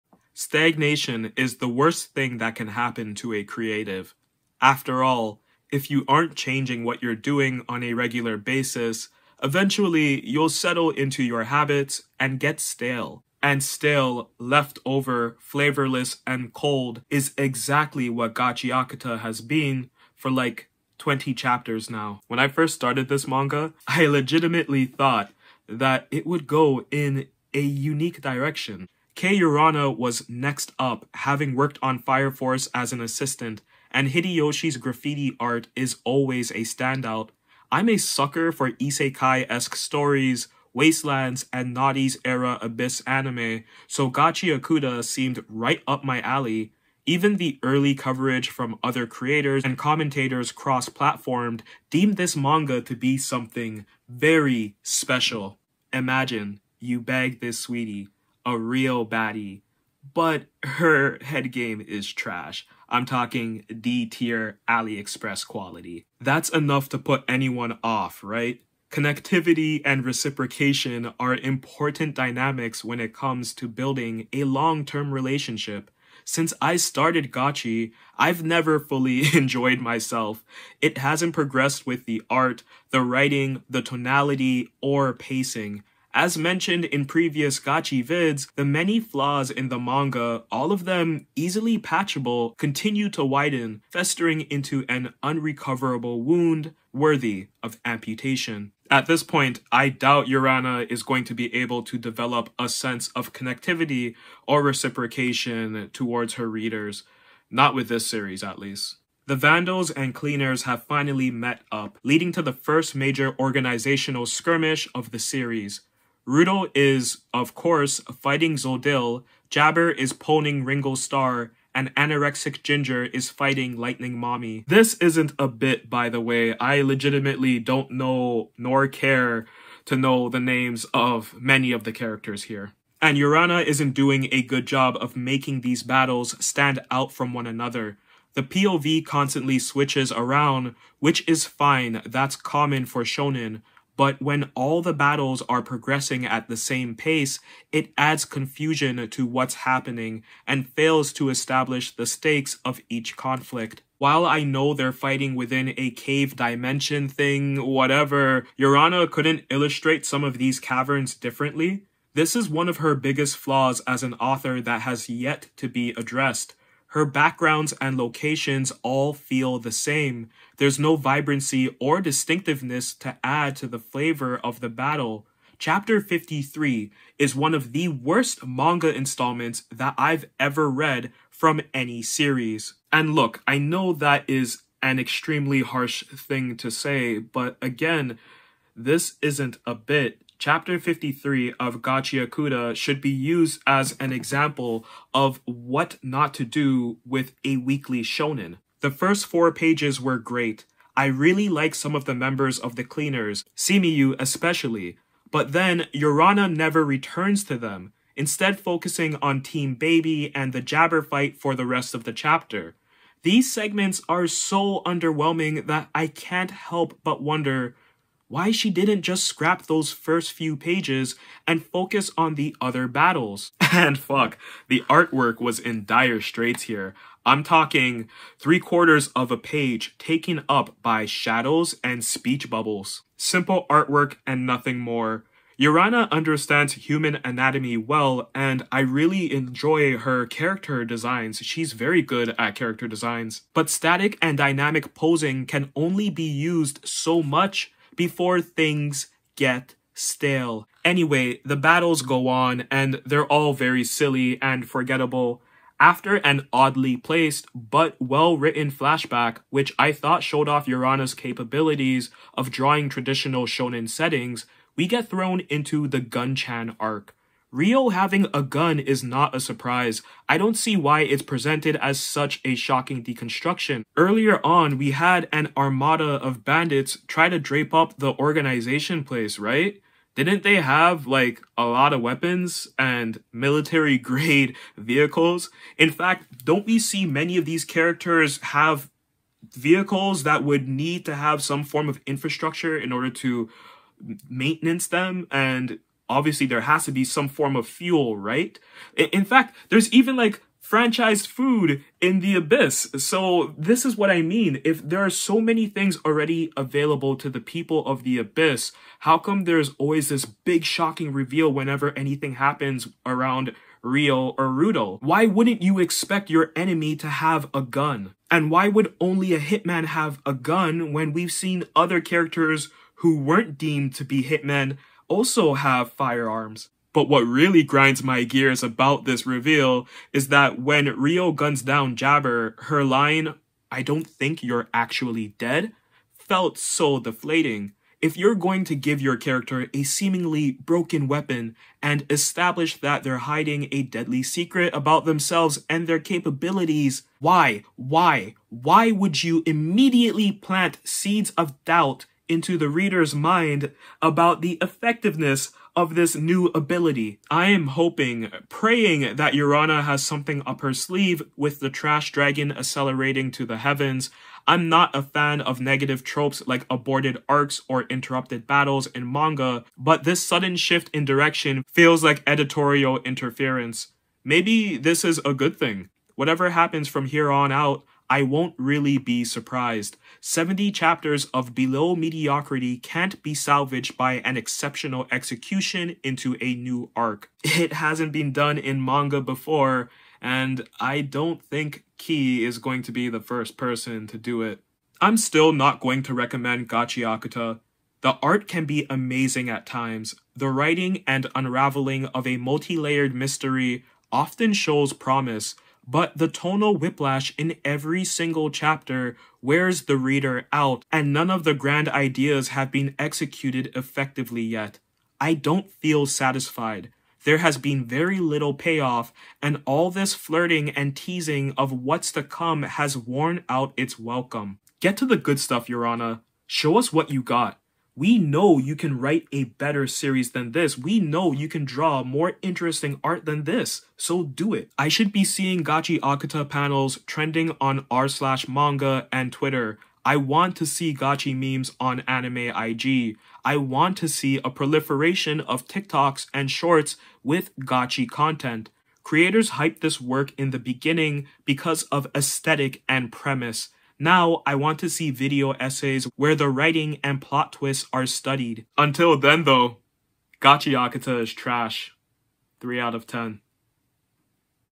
Gachiakuta Is Trash
I will say, Urana gets me to smile at least once per chapter nowadays. But Gachiakuta is still mid/below-mid battle shonen garbage.

3/10. If you're like, really into this sort of manga, go ahead. But literally any other battle shonen out there is leaps and bounds better than Gachi.